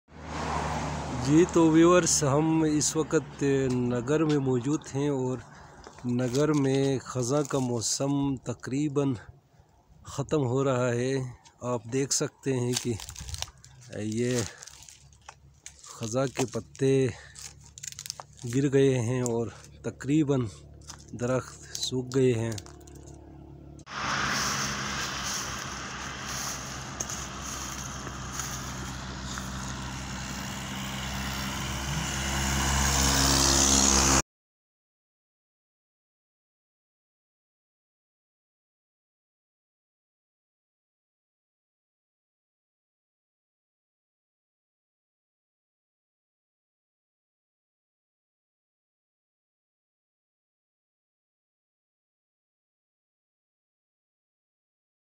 SPLENDOR OF THE SEASON |AUTUMN | FALL LEAVES | NAGAR GILGIT | GILGIT BALTISTAN
Autumn Season starts at Gilgit Baltistan from September and remains till November. Its peak is October 15 to November 15. the end of autumn Season is also looking so beautiful. because the all leaves fall from the tree and scattered in ground. It looks very fantastic. This video also shows the beautiful scenery of ending season at Nagar Gilgit Baltistan.